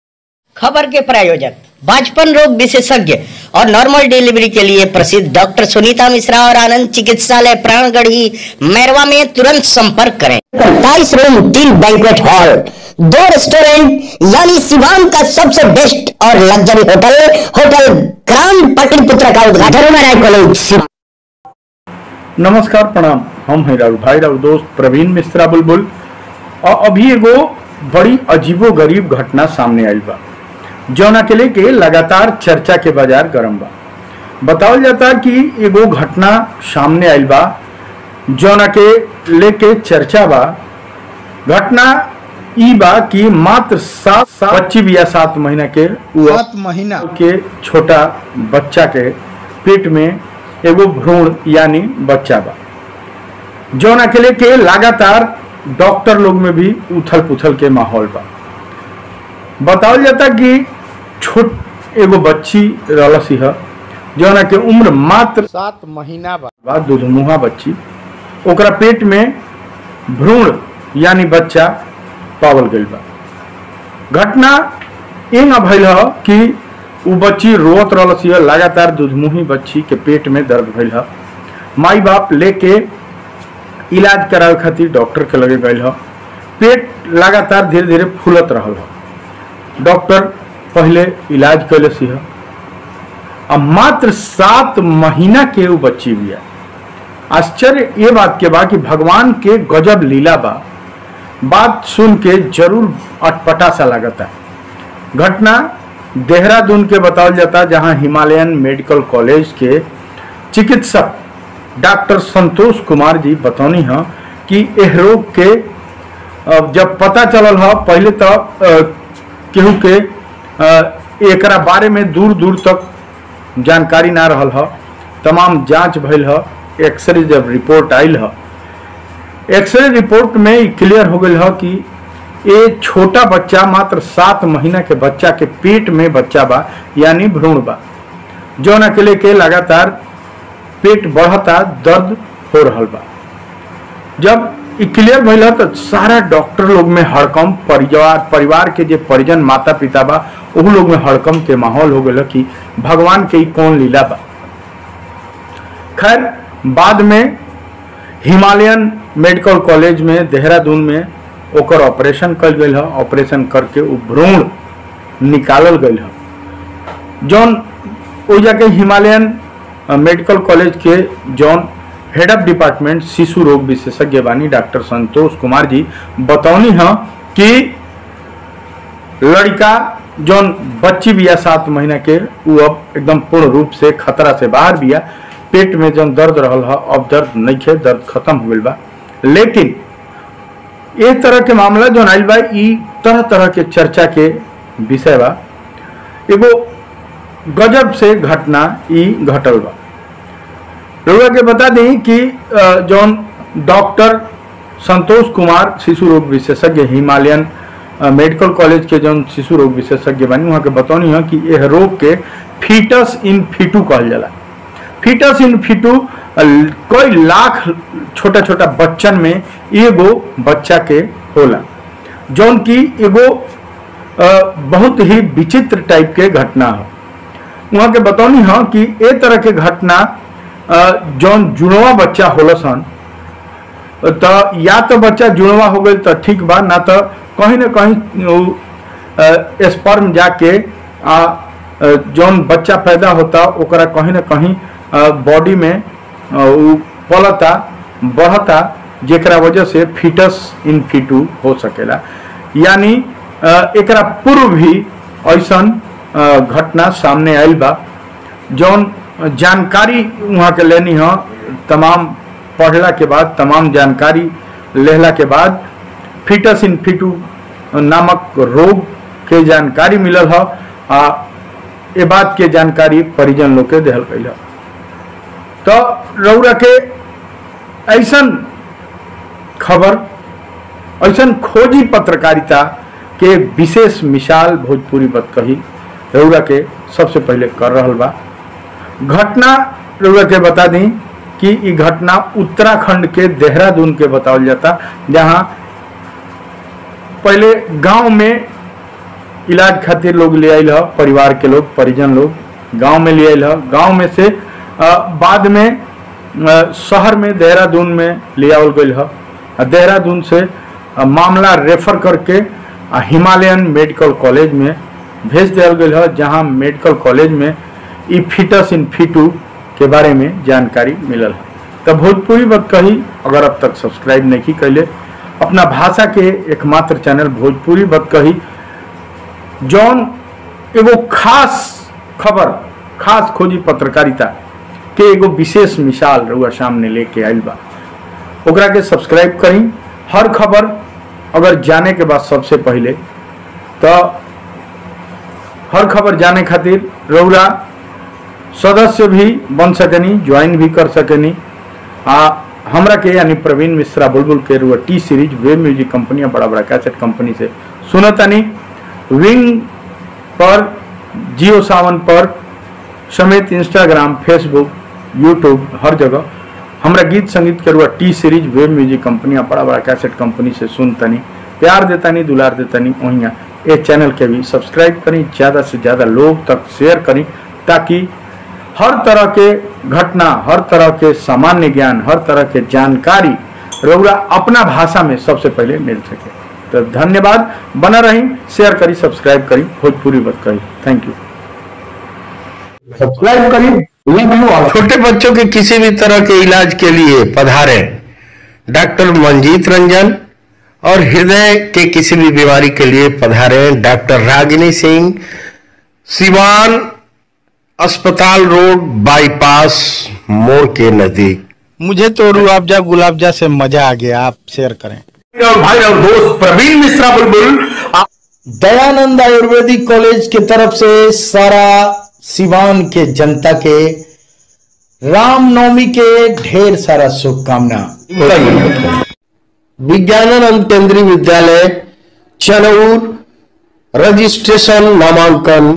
trendingnews #7 माह के दुधमुंही बच्ची के पेट में बच्चा, बड़ी बतकही सनसनीखेज
trendingnews #7 माह के दुश्मनी बच्ची के पेट में बच्चा, बड़ी बतकही सनसनीखेज